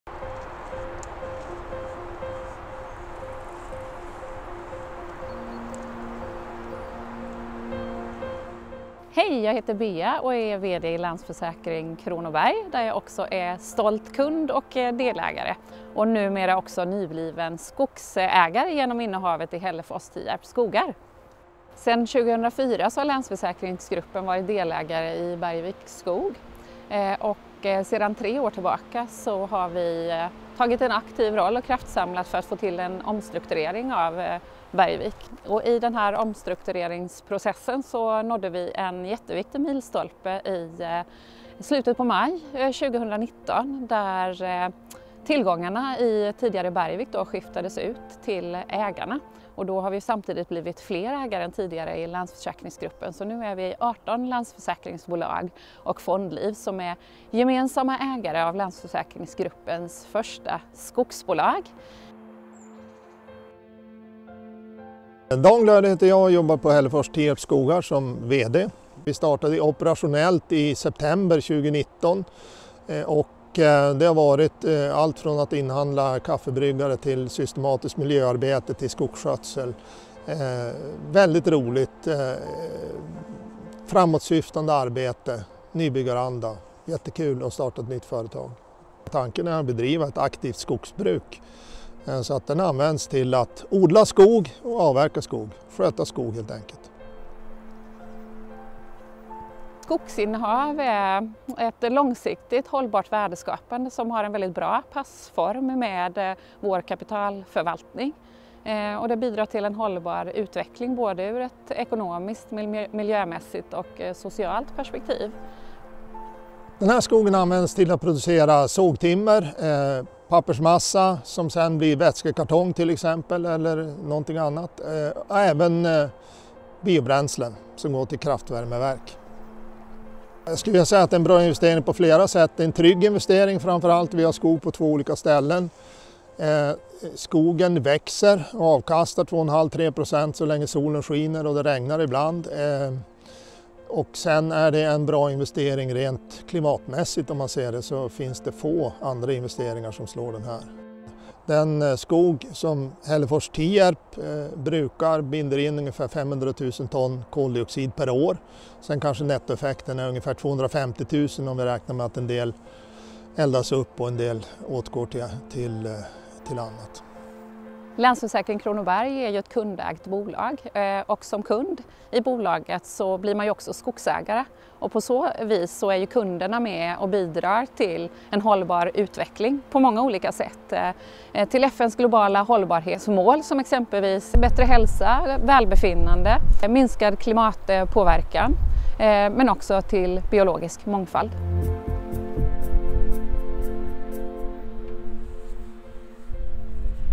Länsförsäkringsgruppen blir skogsägare
18 bolag i Länsförsäkringsgruppen är sedan 2019 gemensamma ägare till Hällefors Tierp Skogar, ett av de största skogsinnehaven i Sverige. Här berättar Länsförsäkring Kronobergs vd Beatrice Kämpe Nikolausson och Hällefors Tierp Skogar Dan Glöde om skogsinnehavet och varför skogen är en historisk hållbar affär.